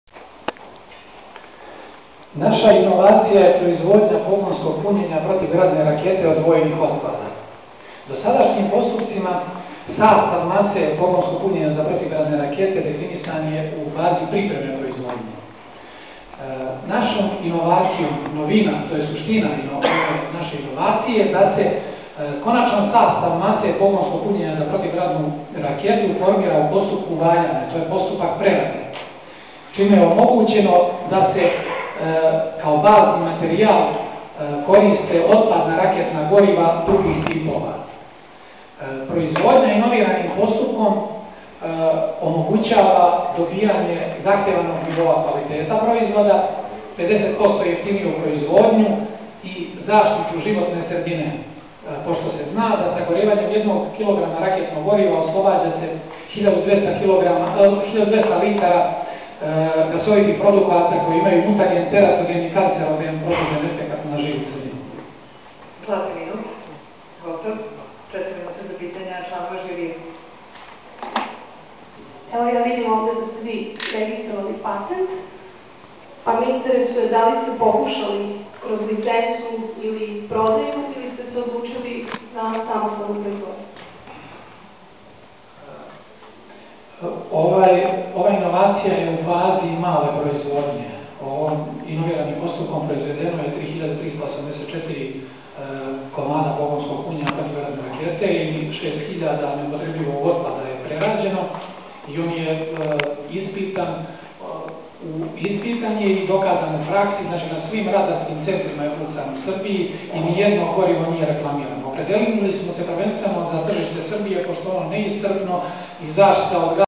TEHNOLOŠKE INOVACIJE - PRIVREDNA KOMORA SRBIJE
INOVACIJA : PROIZVODNJA POGONSKOG PUNJENJA PROTIVGRADNE RAKETE OD VOJNIH OTPADA - Miloš Nešovanović
INOVACIJA JE PO OCENI RECENZENATA I ZIRIJA PROGLASENA MEDU 10 NAJBOLJIH U SRBIJI ZA 2011 GODINU